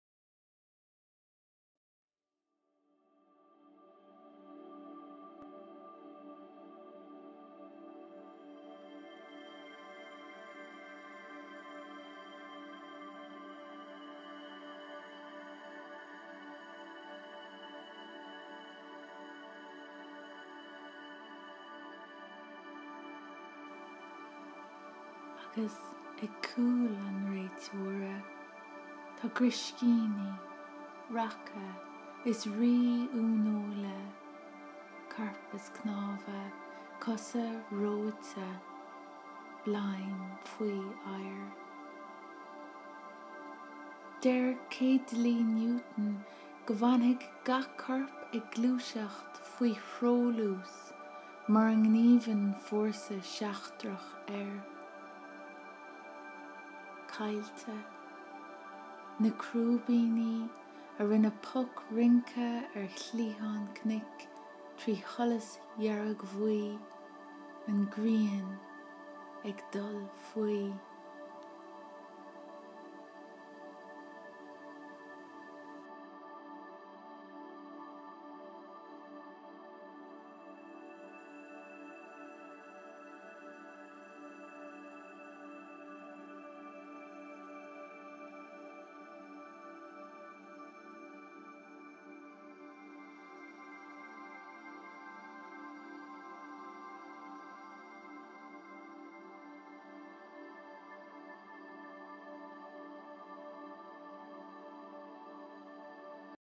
Faoi Mhaighnéidín Cuisneora... // Under a Fridge Magnet... by Doireann Ní Ghríofa
Físdán / Filmpoem le Doireann Ní Ghríofa:

Faoi Mhaighnéidín Cuisneora, Grianghraf de Mhamó mar Chailín Scoile
(Under a Fridge Magnet, a Photo of Grandmother as a Schoolgirl)